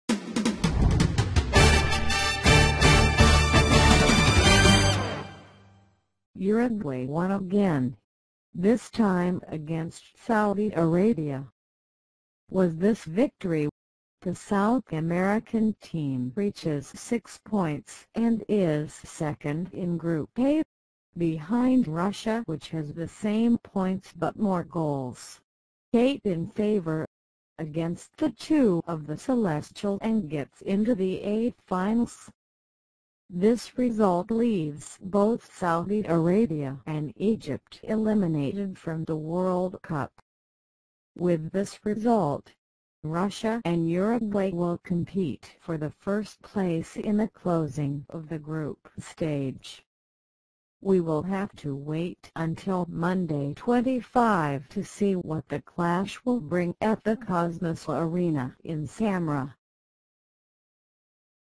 Uruguay wins again (1-0) and gets into the last 16
Uruguay won again, this time against Saudi Arabia. With this victory, the South American team reaches six points and is second in Group A, behind Russia (which has the same points but more goals: eight in favor, against the two of the celestial) and gets into the eighth finals. This result leaves both Saudi Arabia and Egypt eliminated from the World Cup.
With this result, Russia and Uruguay will compete for the first place in the closing of the group stage. We will have to wait until Monday 25 to see what the clash will bring at the Cosmos Arena in Samara.